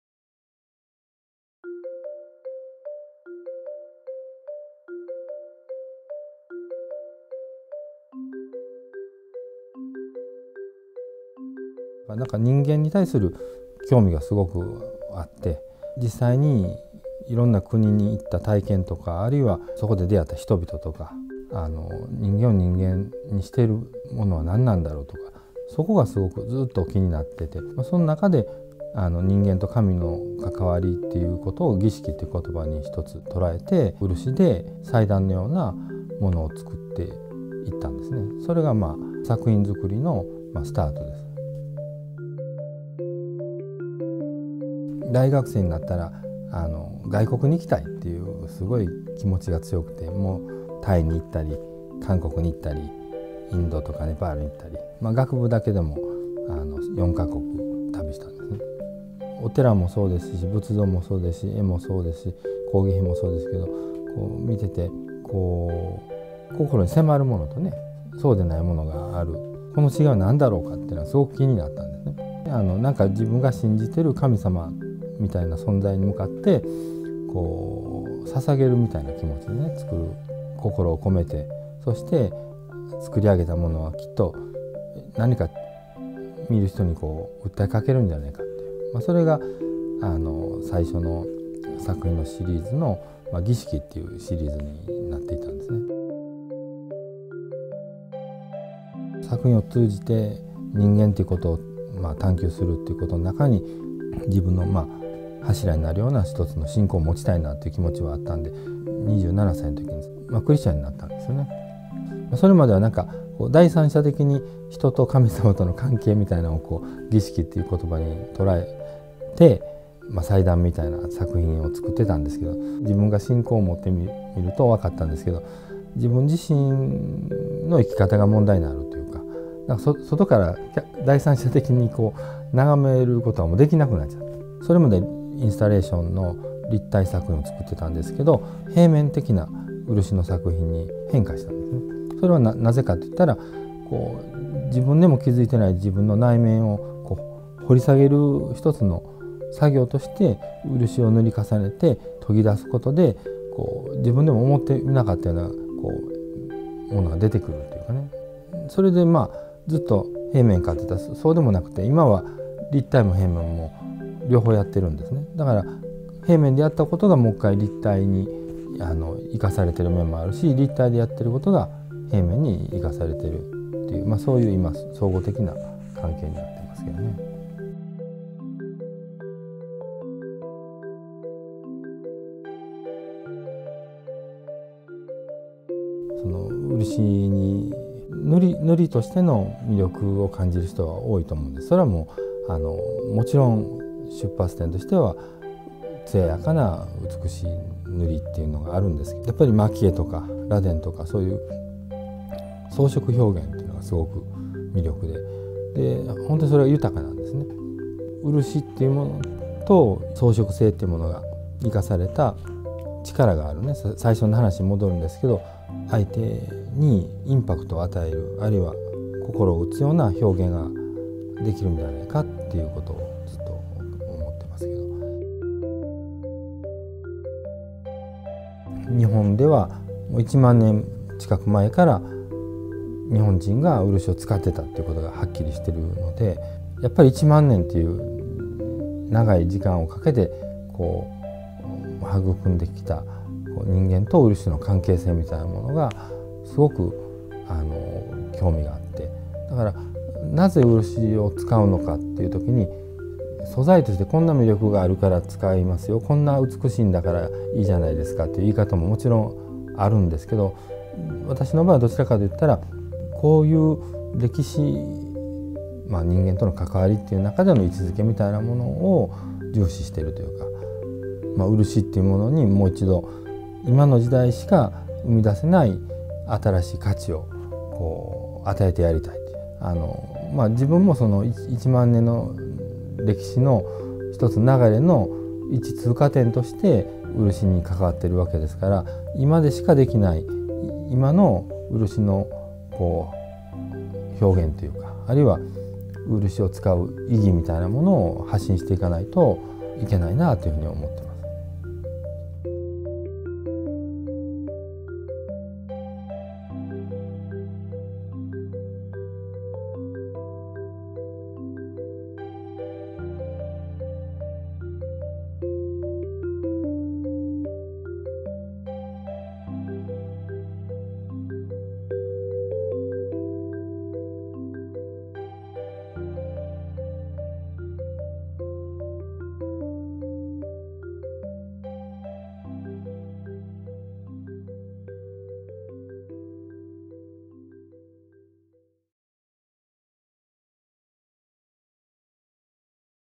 URUSHI Rebirth by KURIMOTO Natsuki
Japanese lacquer Master Natsuki Kurimoto explains the origin of his art
Courtesy of Keiko Art International